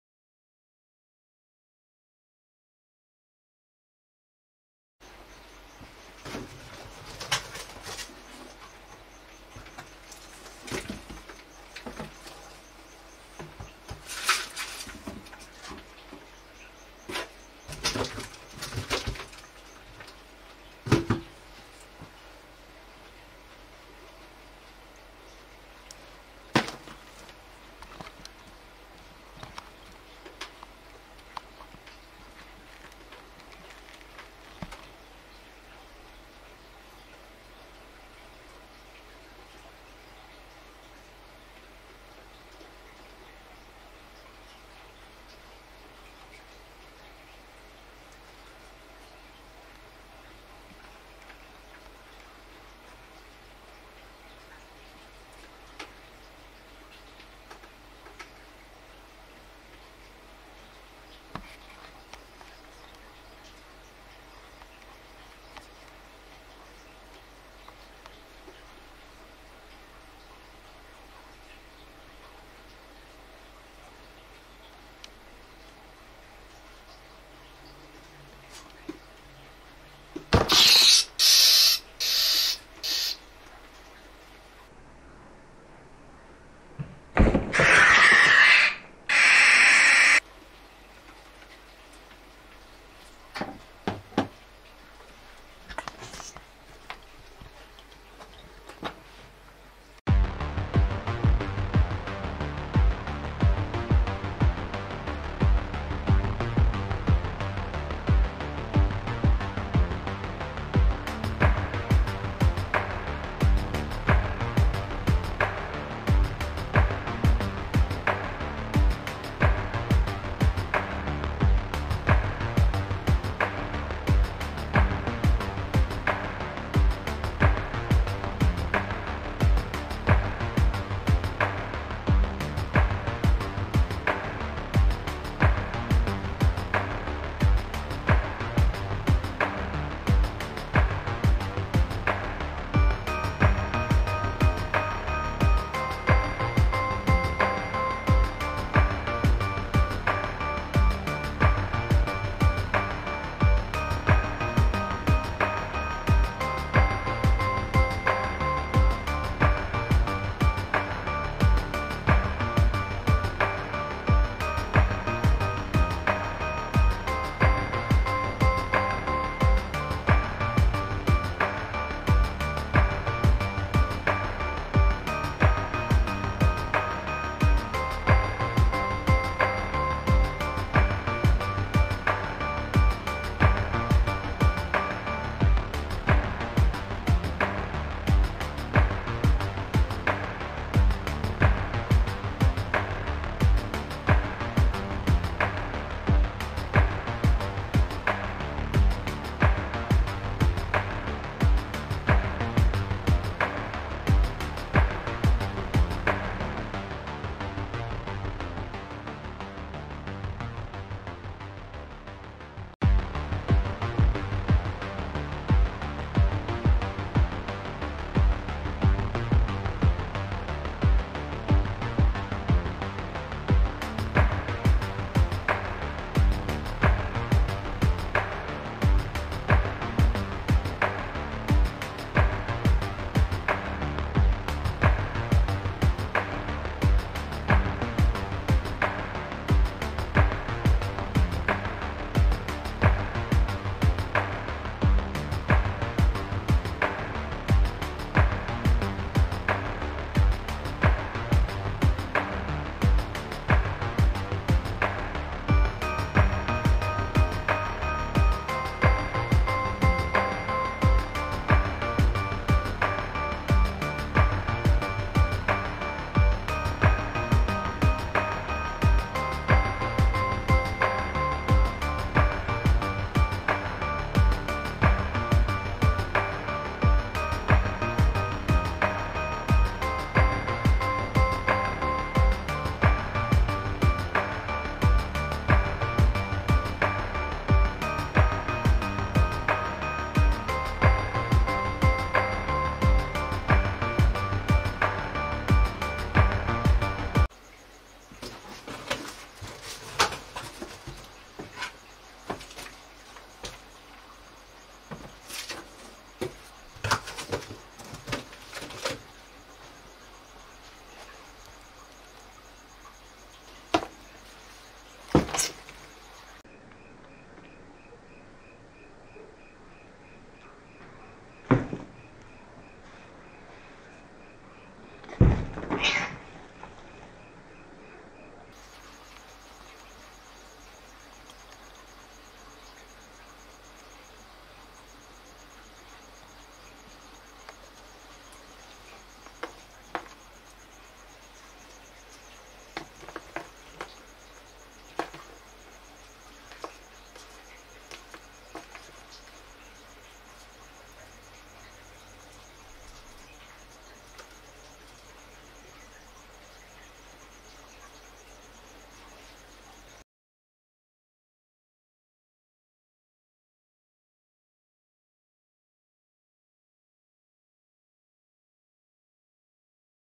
The Pied Python Catches Rats/ Warning Live Feeding
in this video we document a ball python catching and coiling around 1 rat before consuming it, and takes down a 2nd .